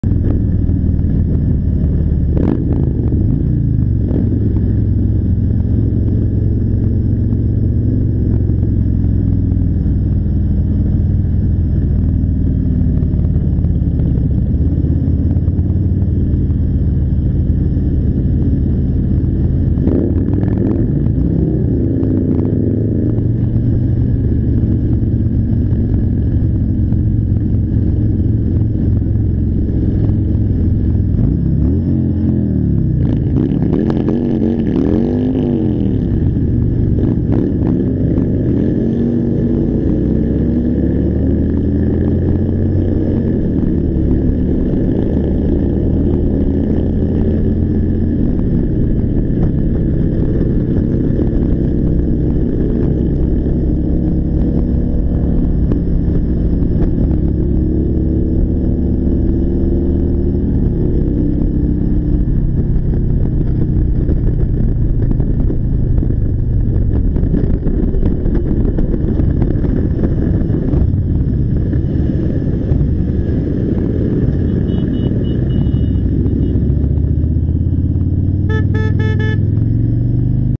Nocna Jazda
Środowe spacery wieczorowo/nocne stają się naszą tradycją 😉😉👍
Kamera nie oddaje nawet połowy emocji i tej drzemiącej w silnikach mocy 🔥🔥🔥🔥
Akumulatory nasze naładowane 🔥🔥🔥